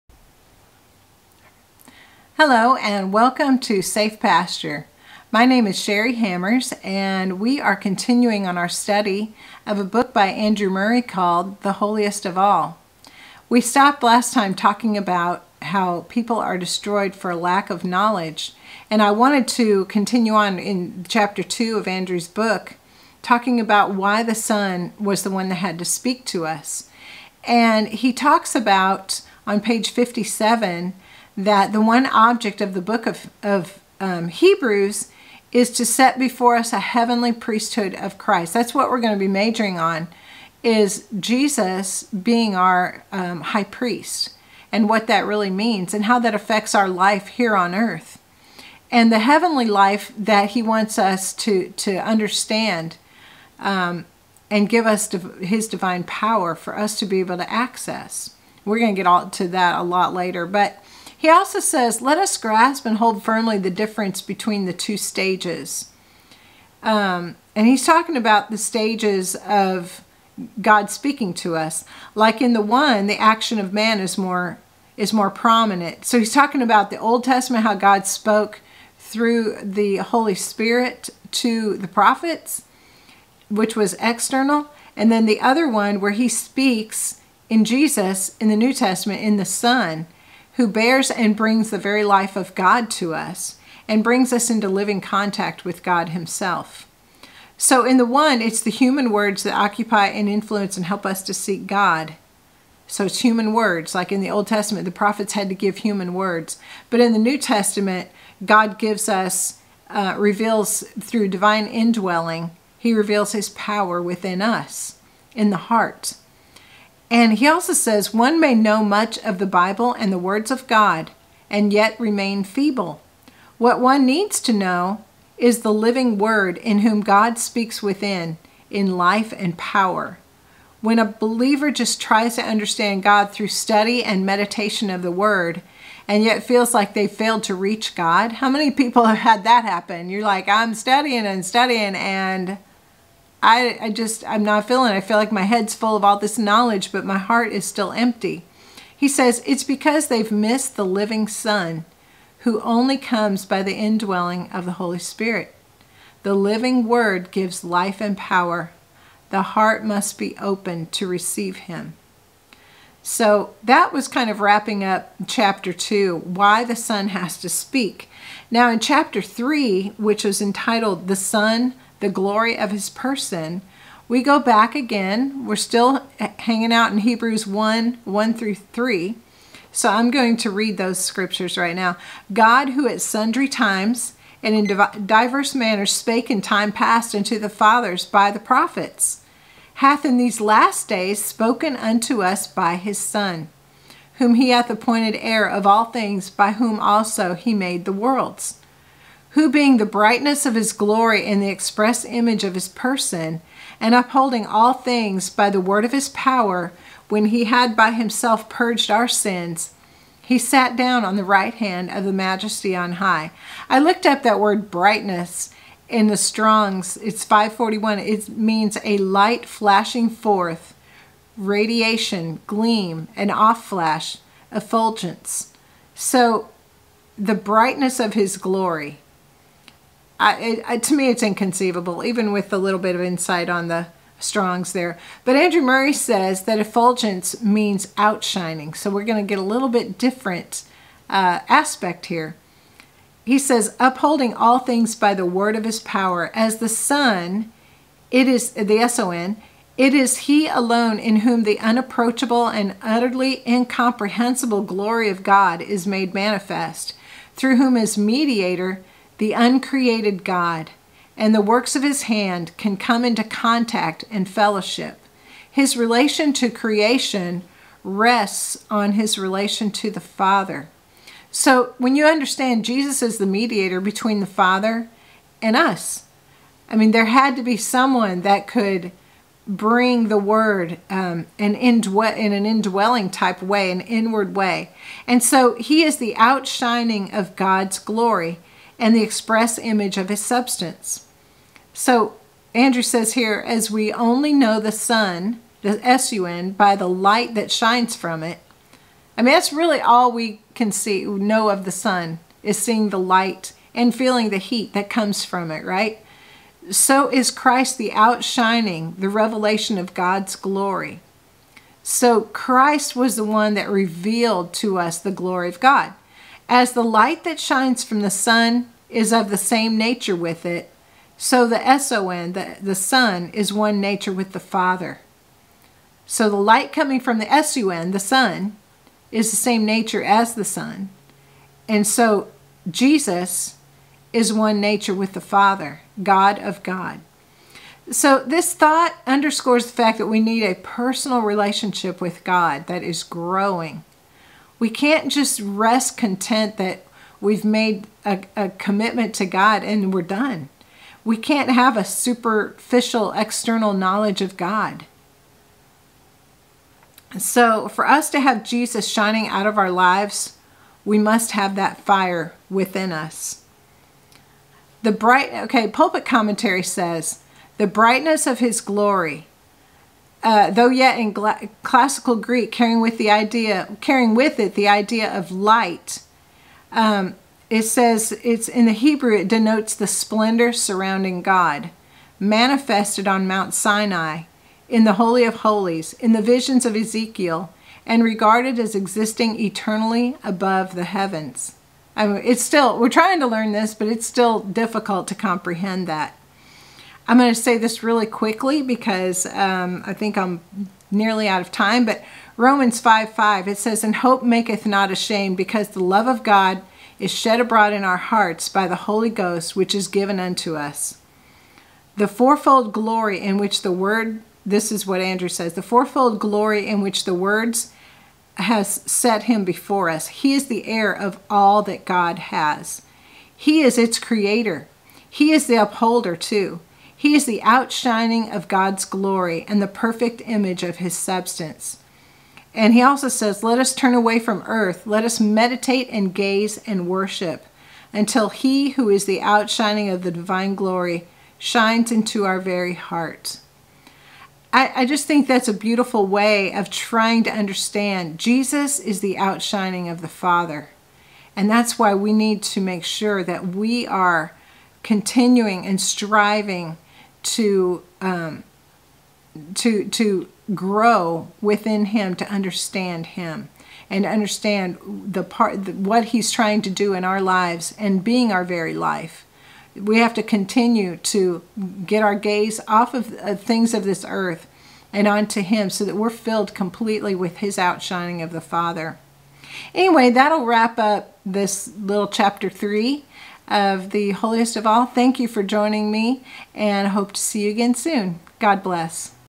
The Son--the Glory of His Person (ch. 3)
The sun has glory, light, & fire.  How does the nature of the sun help us to comprehend the nature of the Son of God?